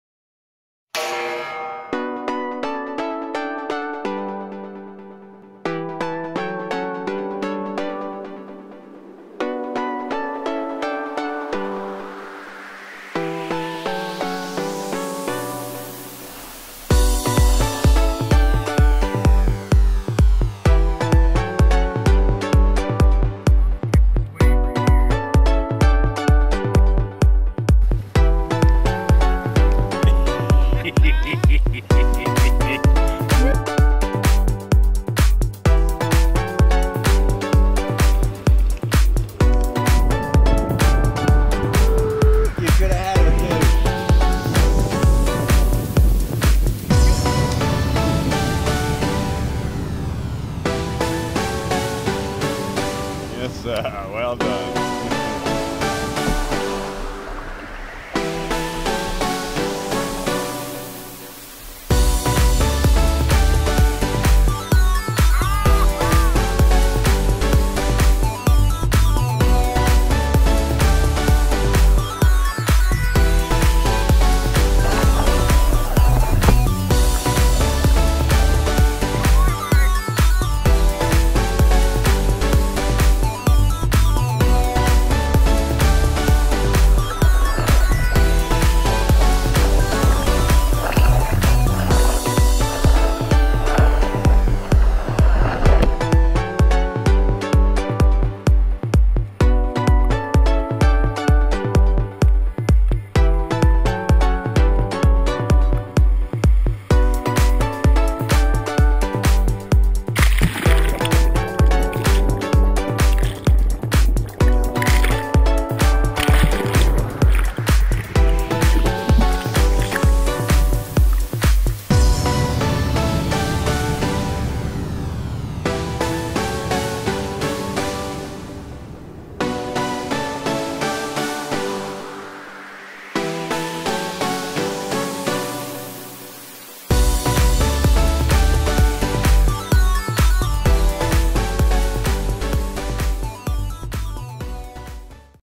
Rock Hopping
Join OutdoorsNB's Jeremy Cline and Dan Smith as they practice some mild rock hopping in the Bay of Fundy near Musquash head.